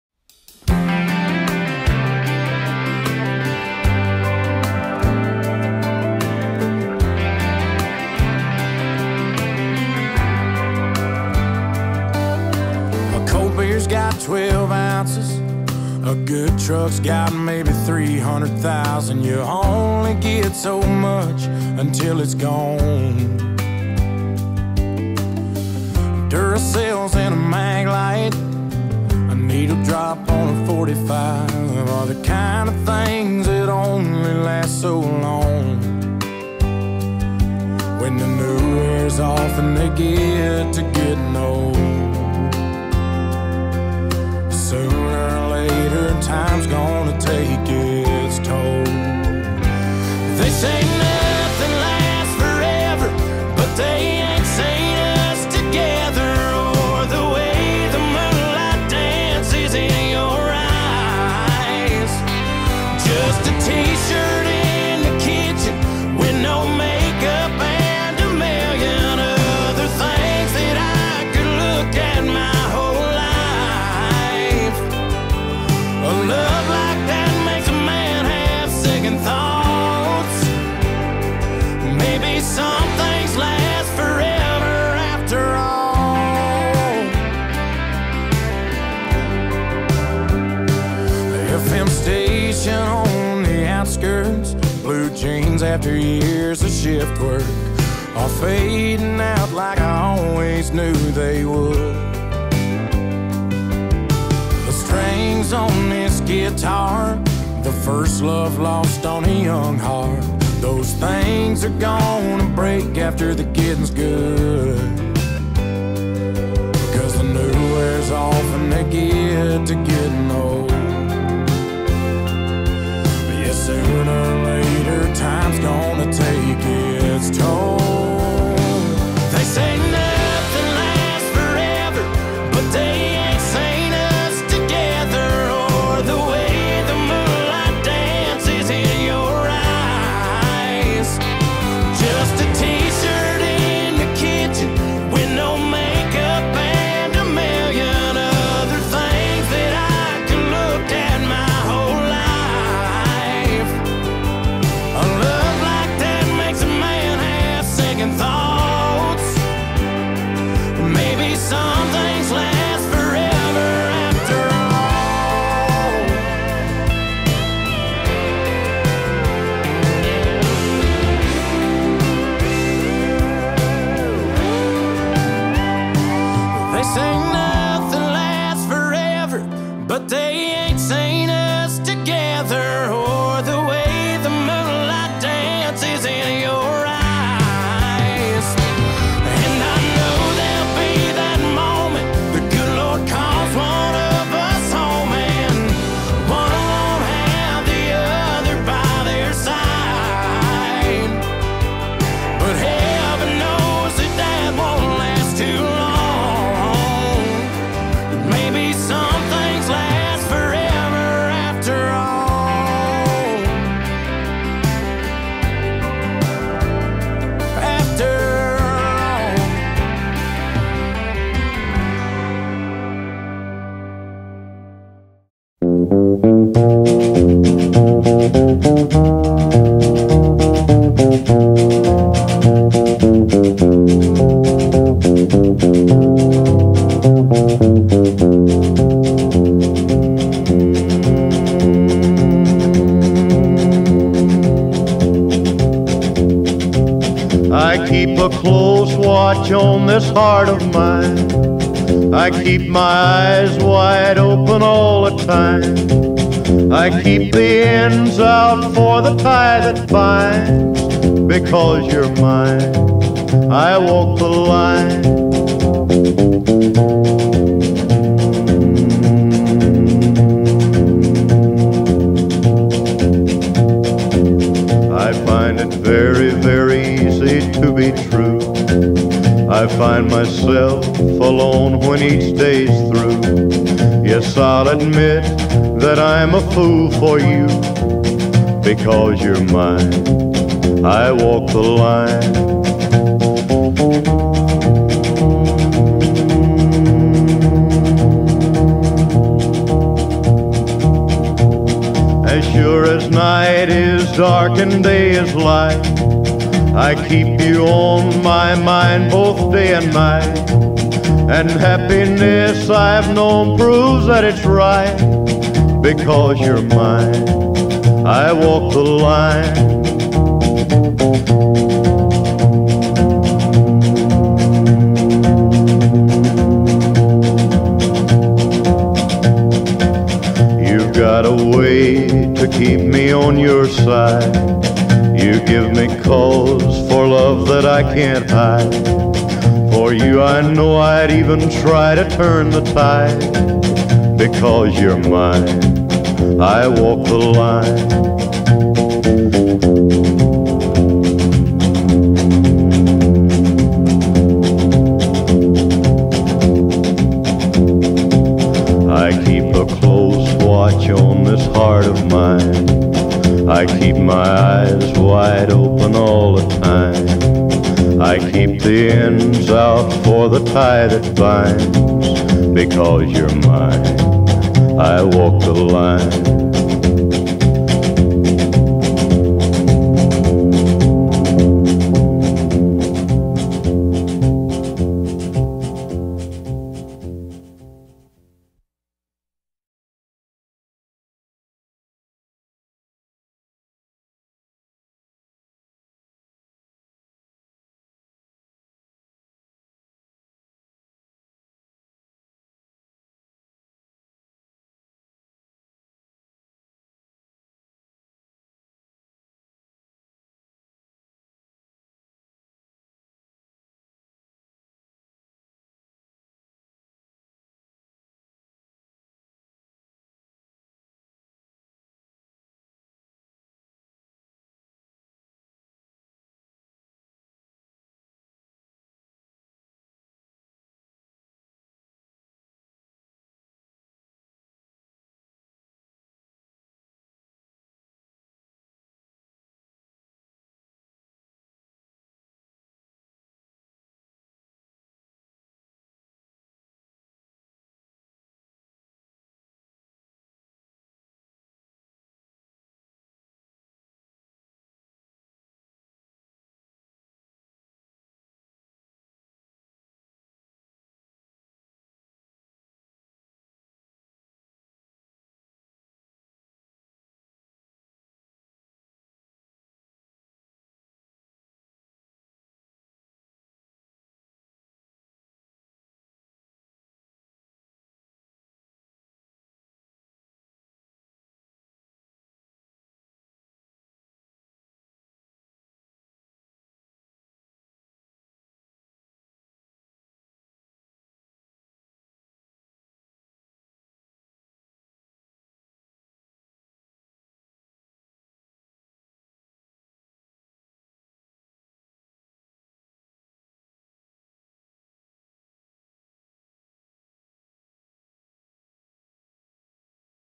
Boots 2020 APHA Sorrel Overo Gunnatrashya gelding
This fancy chromed out gelding will be offered April 6th at the Heber Valley Horse Sale